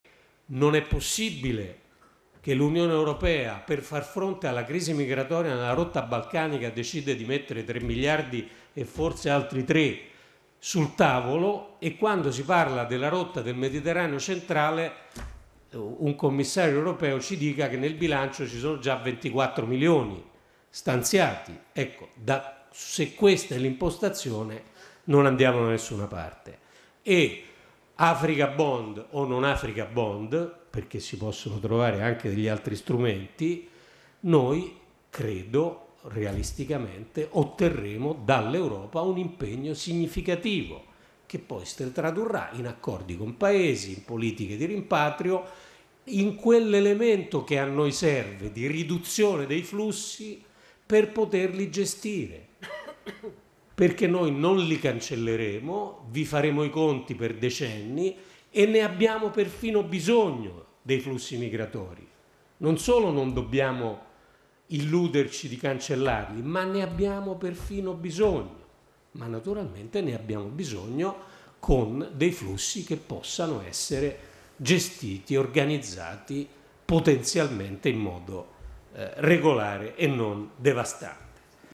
Gentiloni: UE, abbiamo bisogno di flussi migratori ma non gestiti in maniera devastante
(Agenzia VISTA) - Roma, 3 Maggio 2016 - "Quale Europa per il XXI secolo?", il convegno presso la Camera dei Deputati organizzato dal Partito Democratico.